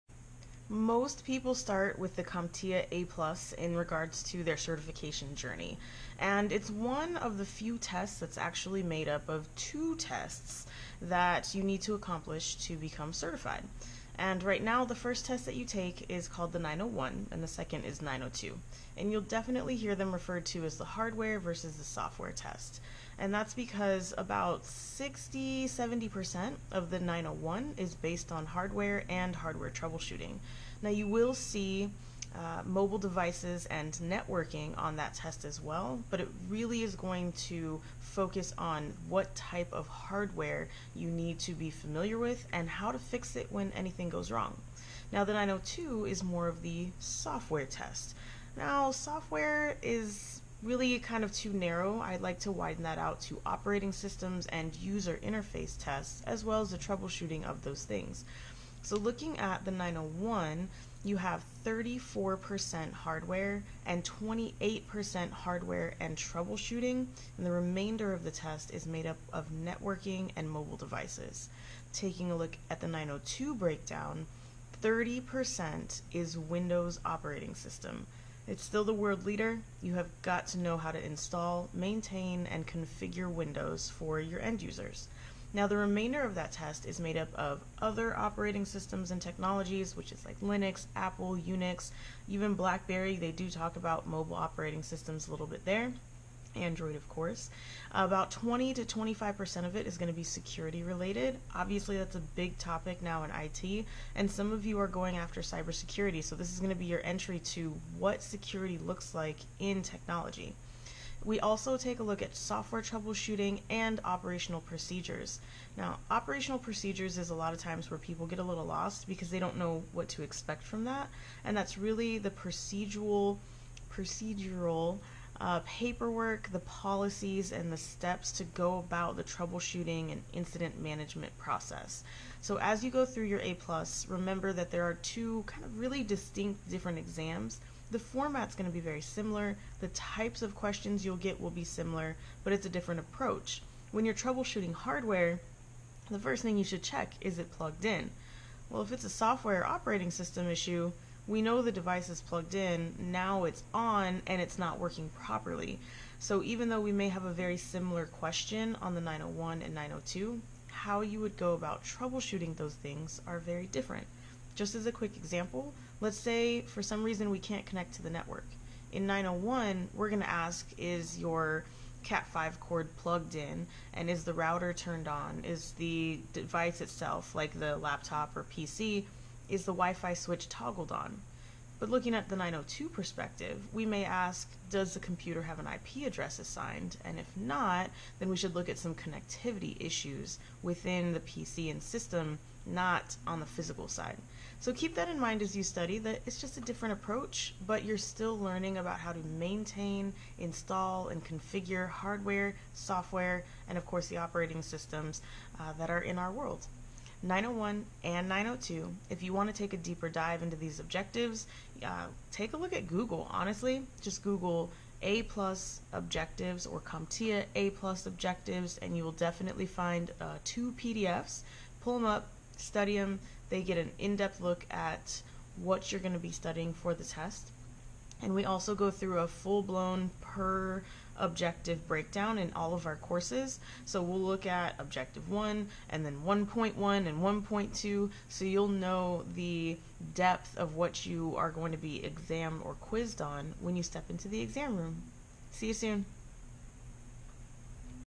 CompTIA A+ Exam Objectives Review
A summary review of the 901 and 902 exam objectives.  Hardware, software, troubleshooting and more.